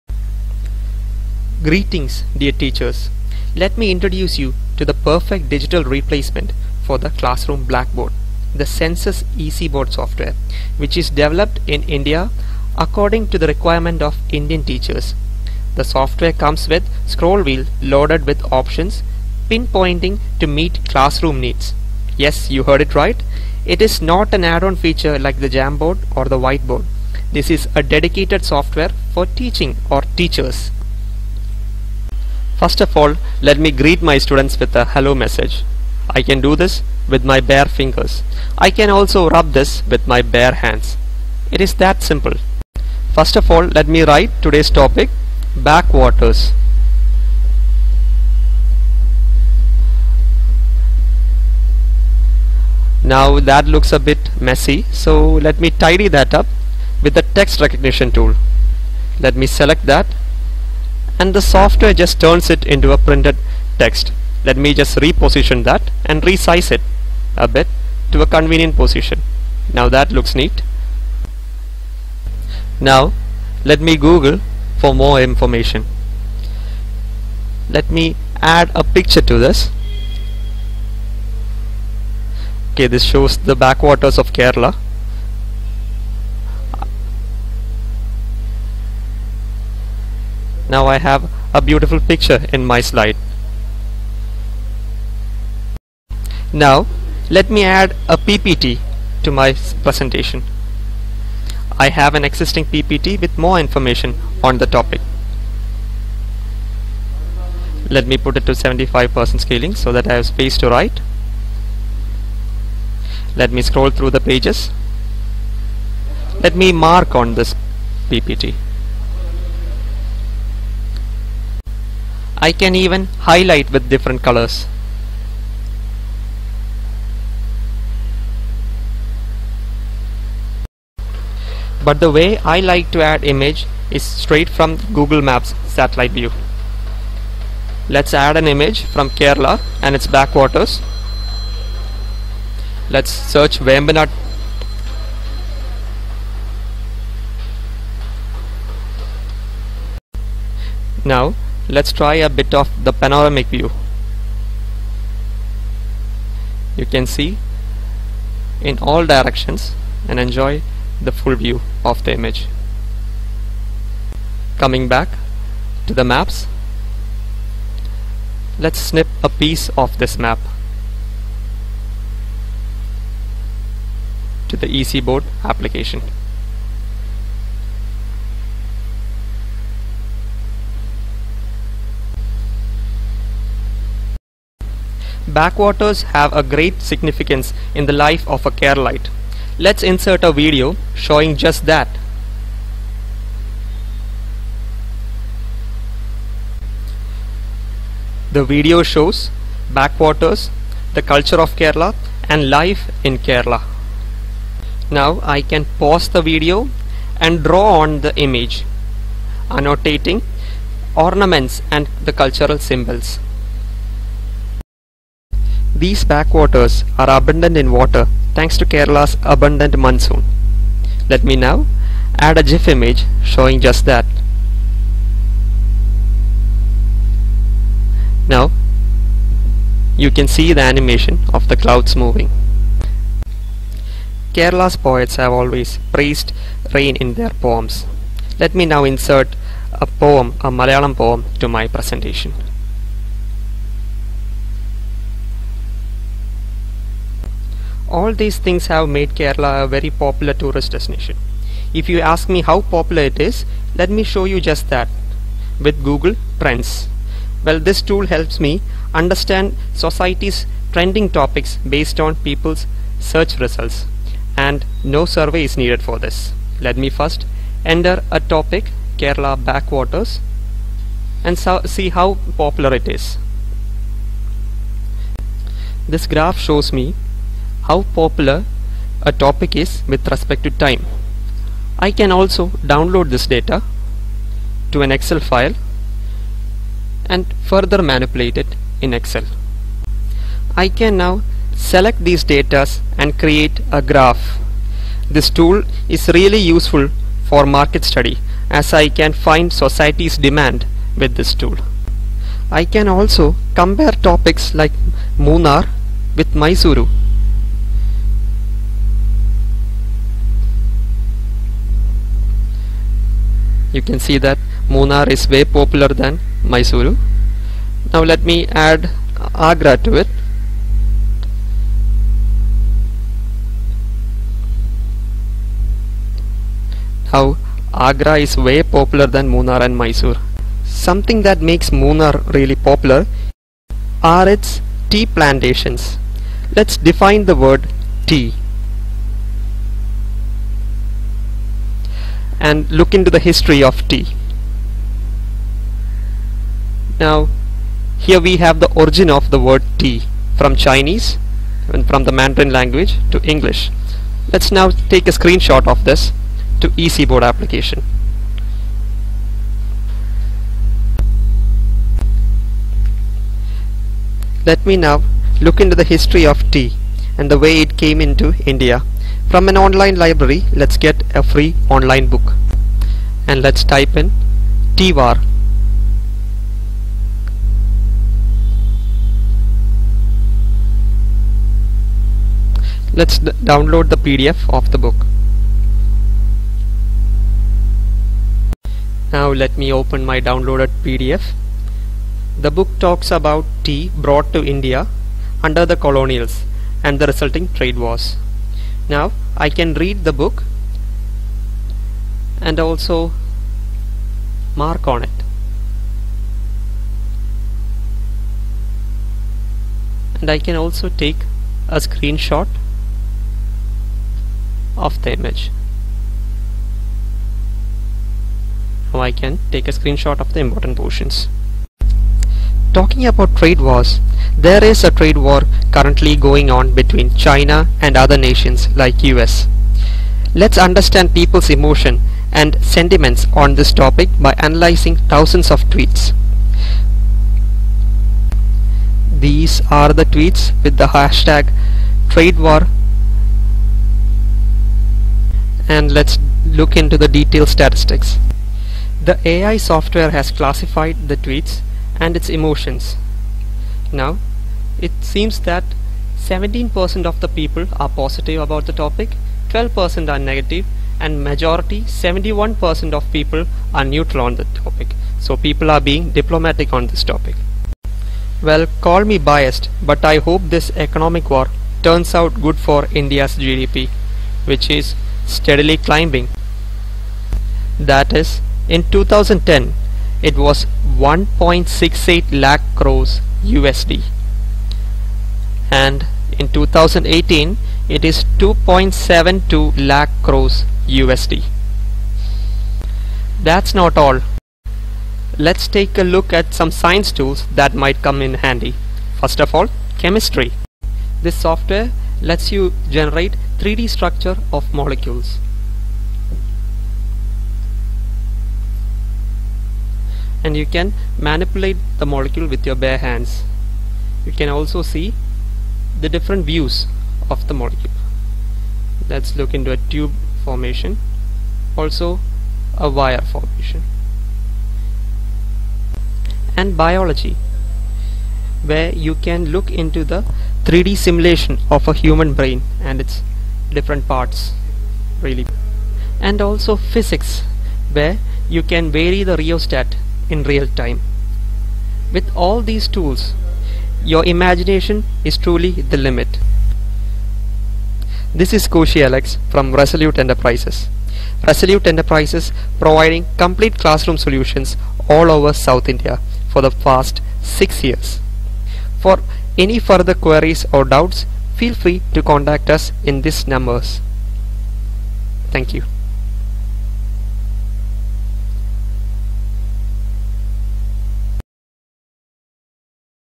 Online class demo on easi board
A very well made demonstration of our EasiBoard features by our Kerala Team. Extremely easy to use software for education that facilitates dynamic learning processes and interactive teaching learning environment.